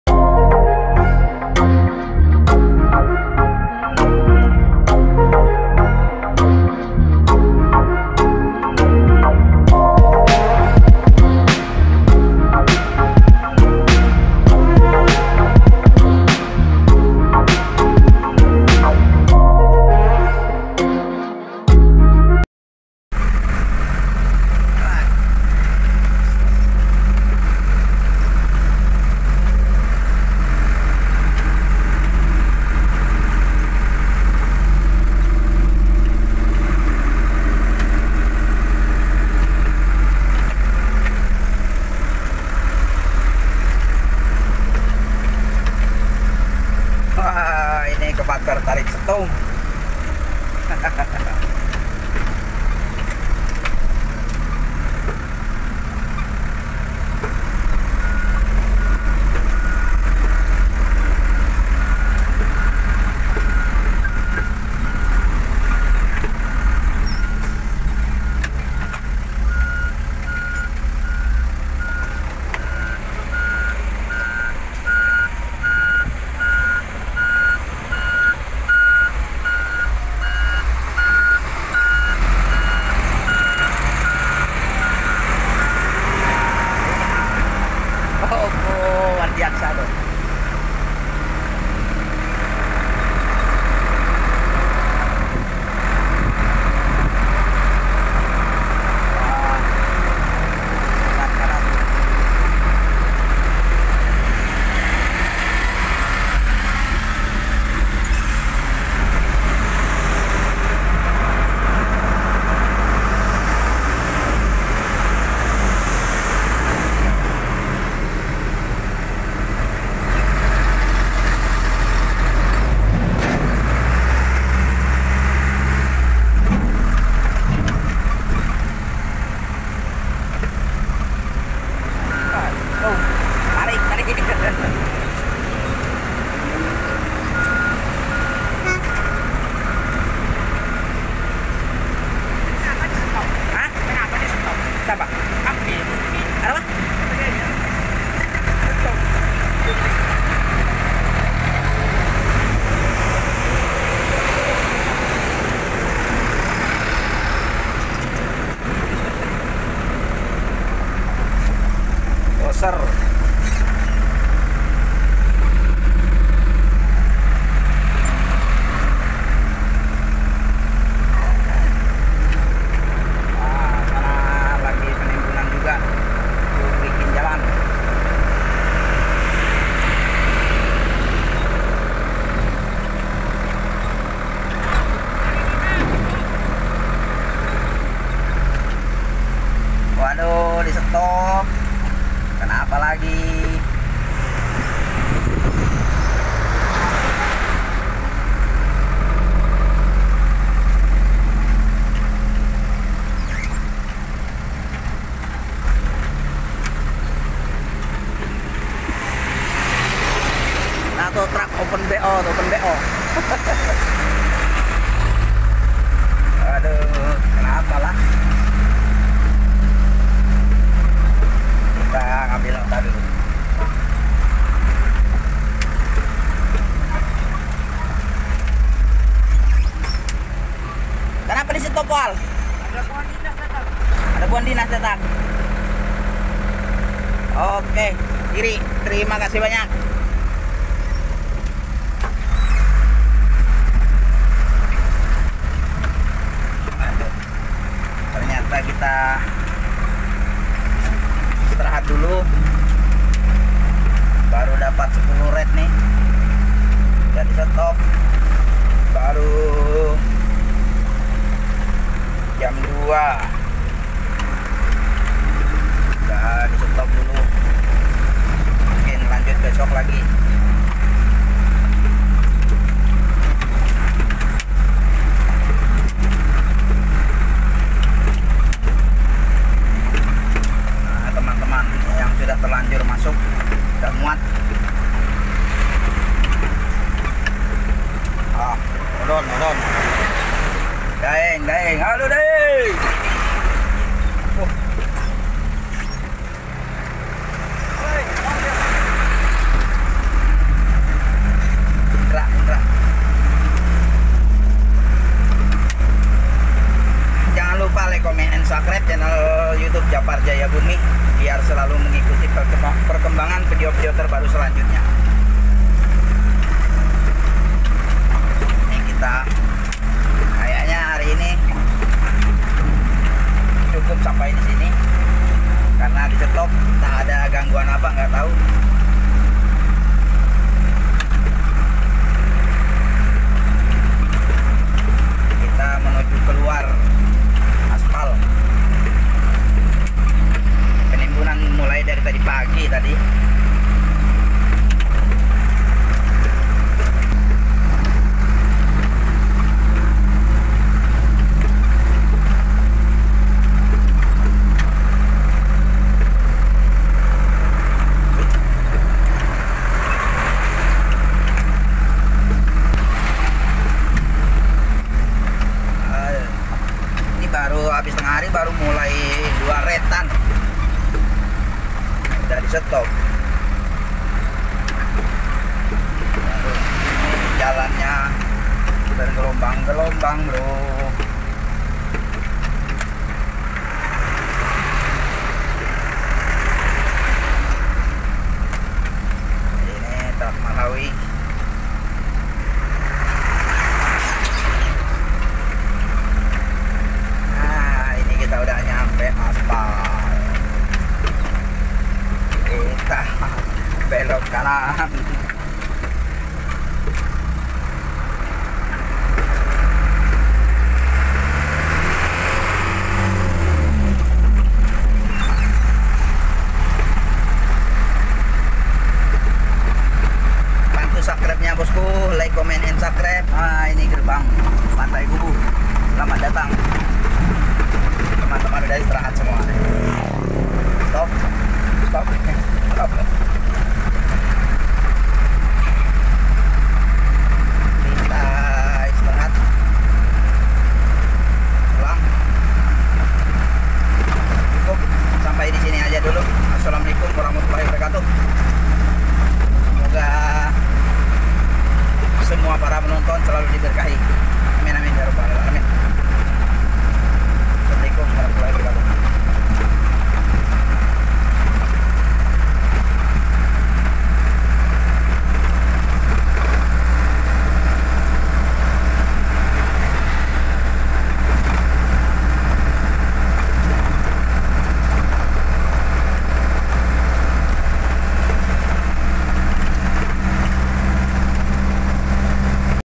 EXAPATOR,SOPEL,BULDOZER,STUM...!!! DI KERAHKAN DEMI KELANCARAN PENIMBUNAN .
salam menonton YouTube JAPAR JAYABOEMI,demi kelancaran dan kenyamanan para sopir truk semua alat berat di pekerjakan/kerahkan agar nyaman fan nyaman saat mau melakukan pembongkaran tanah uruk .